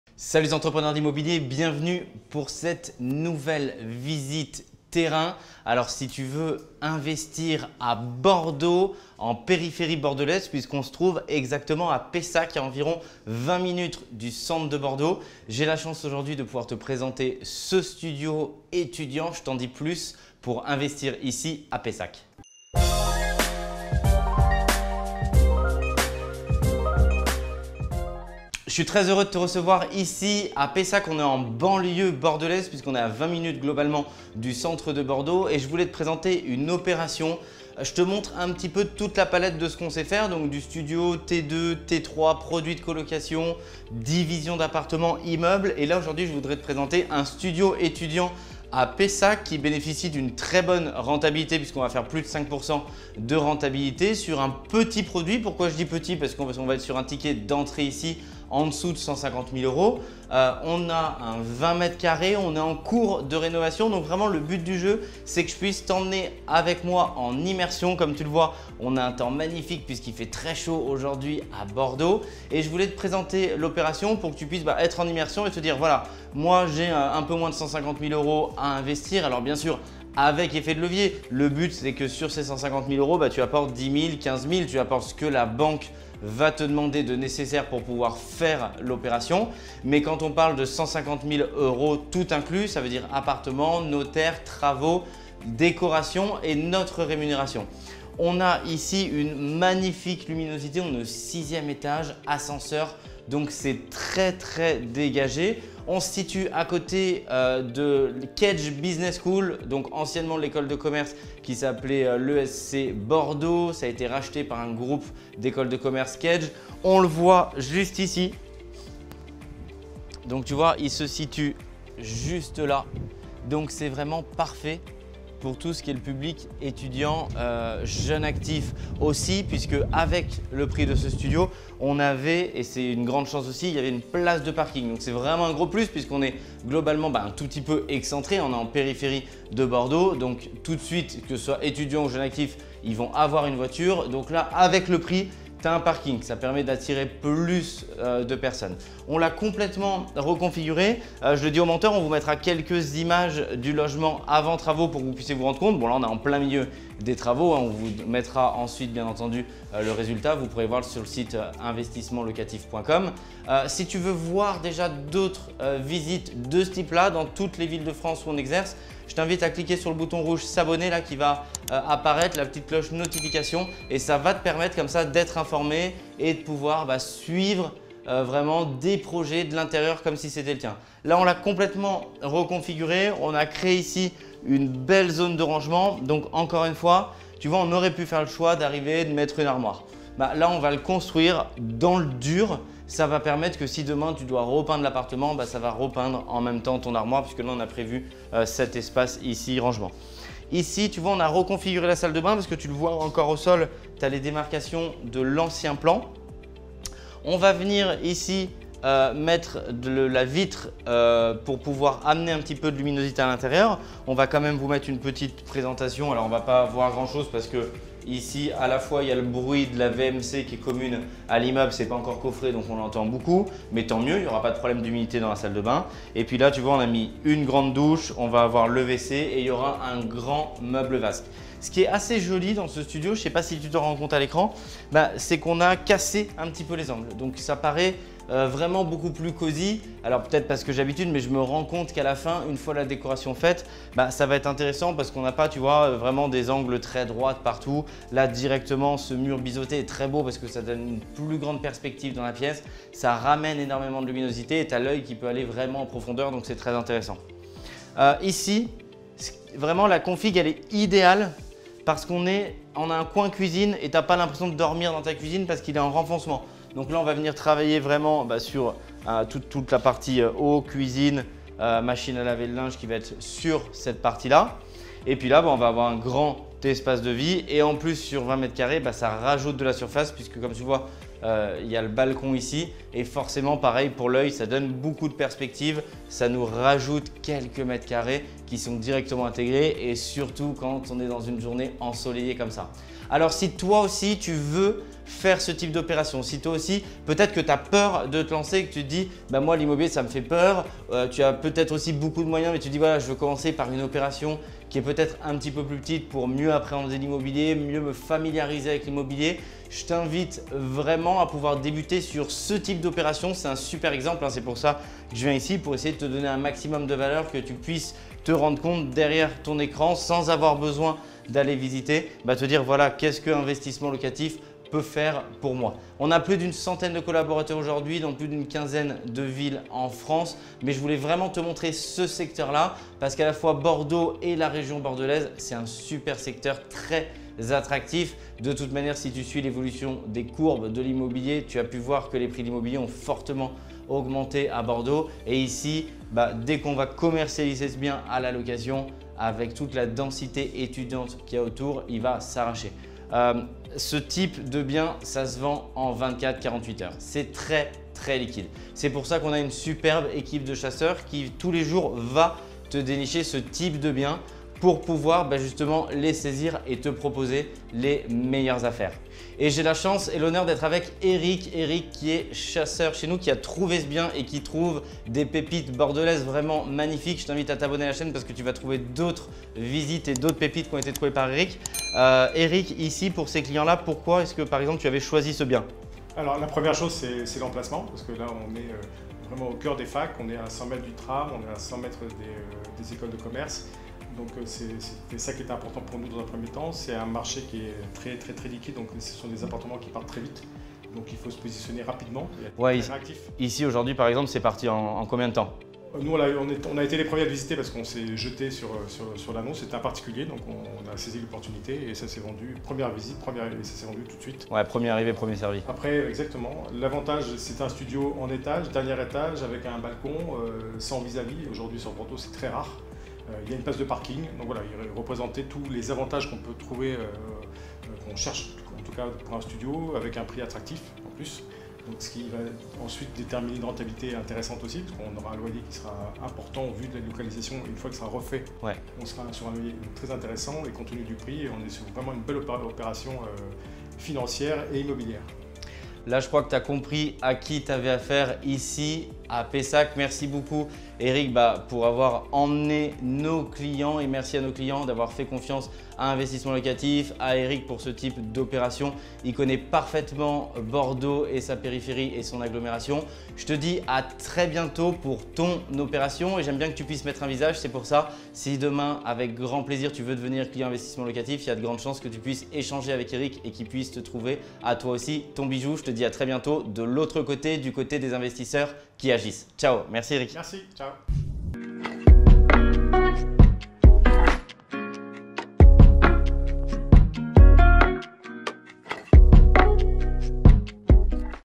Opération De Rénovation Immobilière À Bordeaux !
Dans le vidéo du jour, nous partons à la visite d'une opération de rénovation immobilière à Bordeaux. L'emplacement de ce bien est un plus pour sa rentabilité. Cet appartement est situé au 6ème étage avec ascenseur, bien lumineux juste à coté d'une université.

Ce studio bénéficie d'une place de parking puisque c'est une zone situé dans la banlieue Bordelaise. Je vous fais la visite de ce bien. Découvrez dans cette vidéo une opération de rénovation immobilière à Bordeaux.

Bon visionnage.

0:00 Intro
0:20 Début
1:07 Les atouts de l'appartement
2:09 Privilégier l'emplacement
3:50 La vue depuis la fenêtre